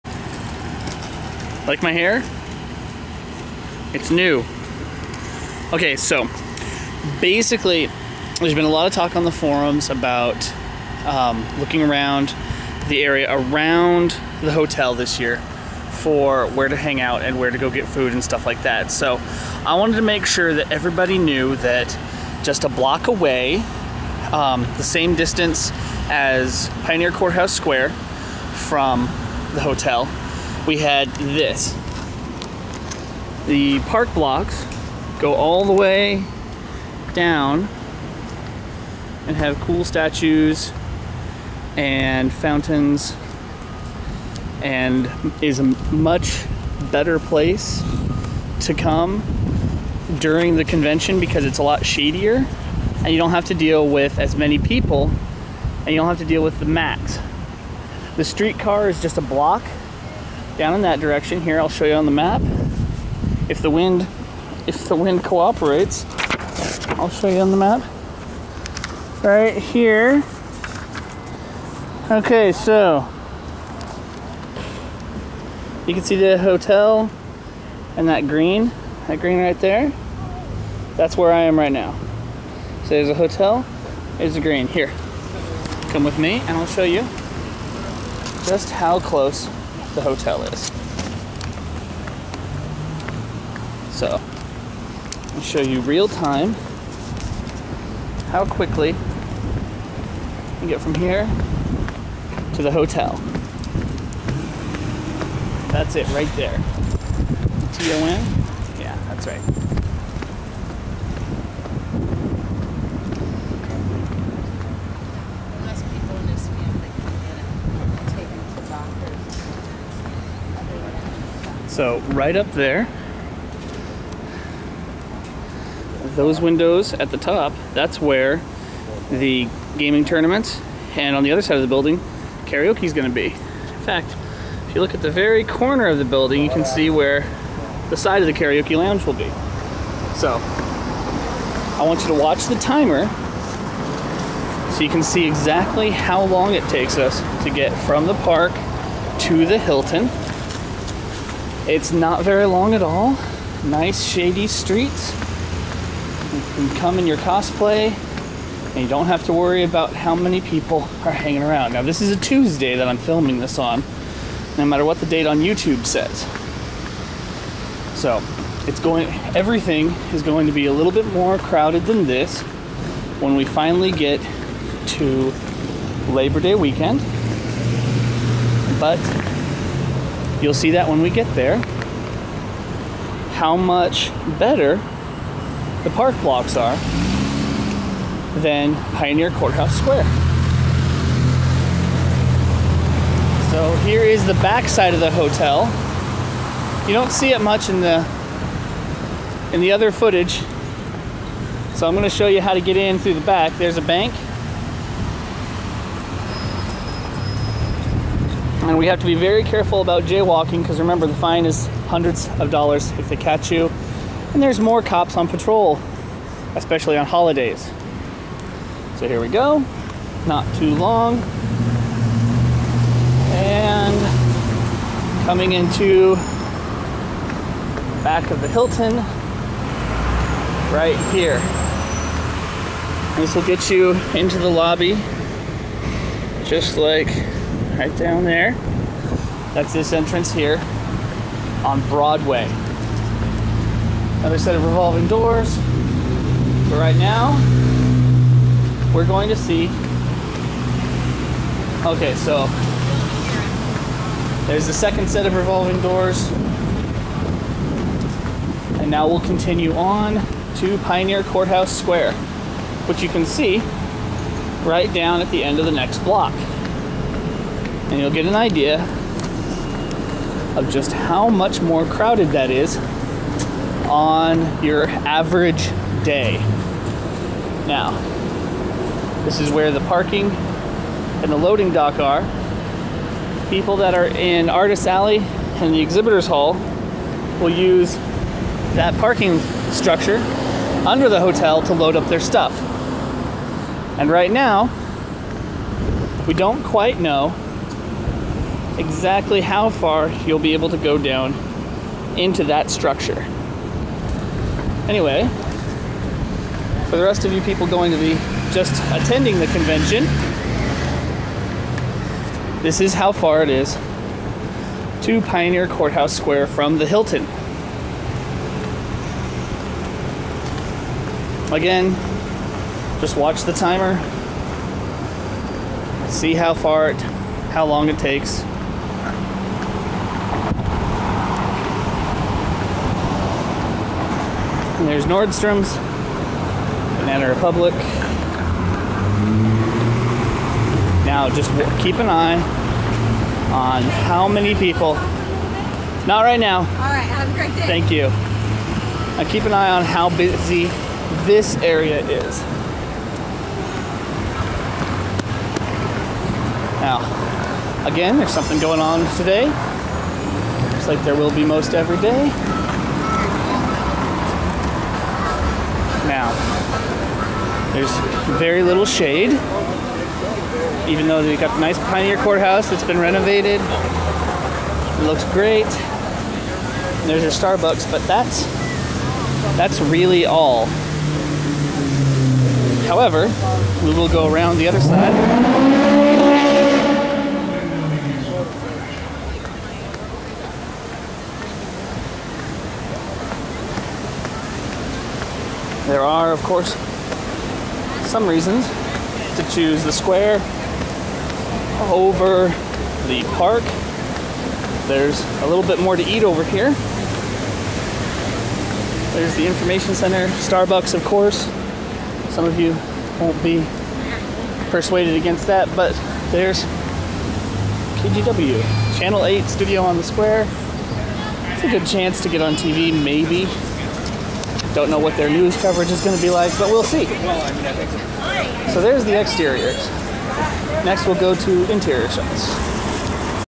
Kumoricon July Recon - Park Blocks vs Pioneer Square
This video was designed to show how close the park blocks and pioneer courthouse square are to the Portland Hilton. It's also designed to show you that the park blocks are a better location if you want to get out of the conspace for a little while and are itching for the "park across the street" that we've had for a few years now.